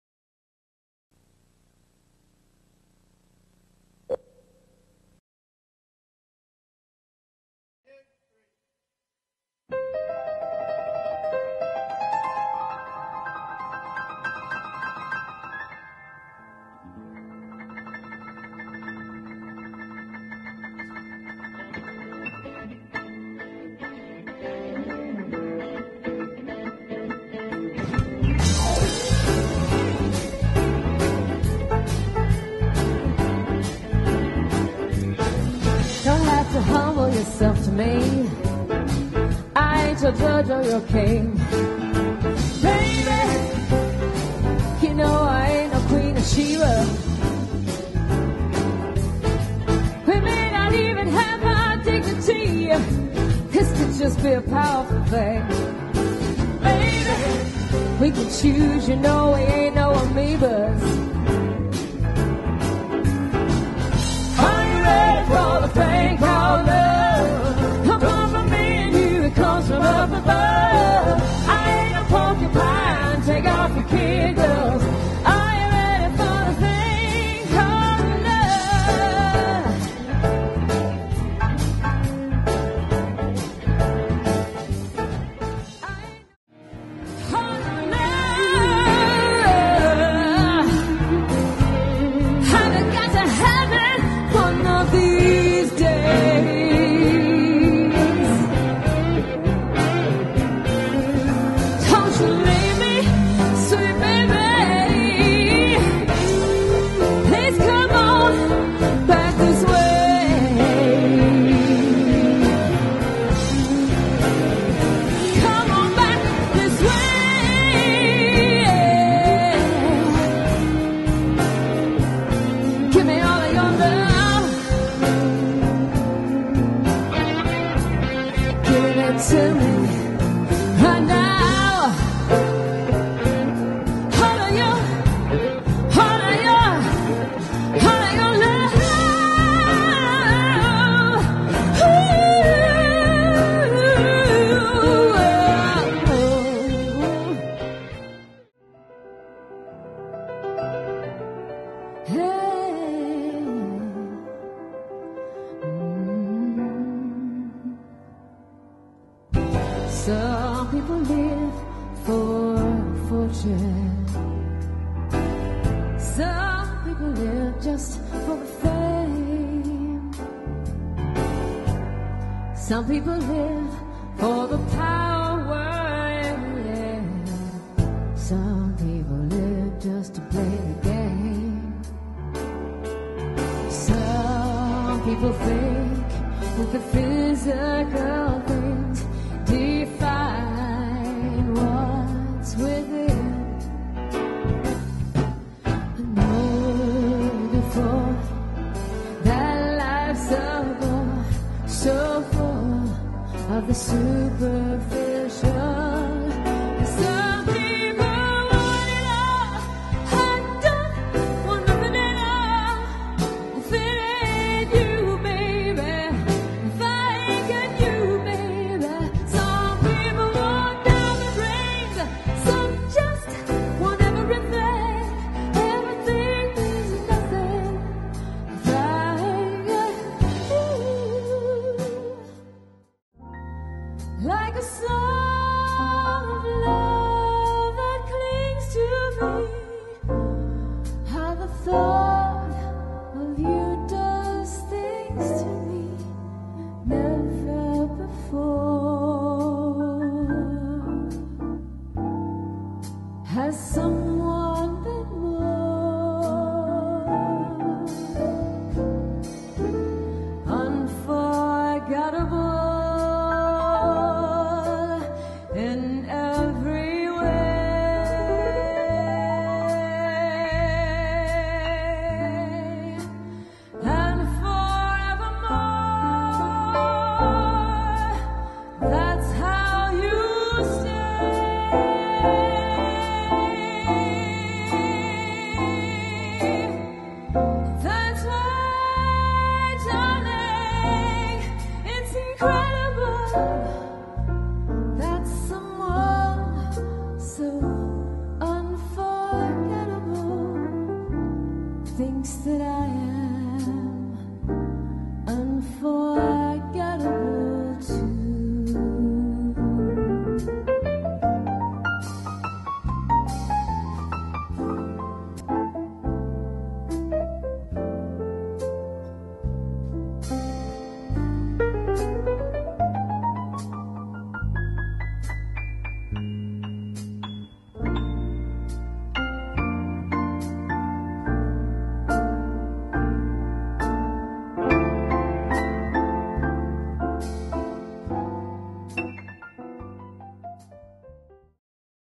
Beth Mankel Promo
Beth recorded this video live at Crossroads Christian in Lexington Ky. Feb 1, 2014.
Thanks to the guys in the band: Mike Mankel, guitar, Preston Gorman, Drums, and William Marshall, Bass. Special thanks to Patrick Drury. Derek Inouye and Coby Gammon for their video and sound expertise!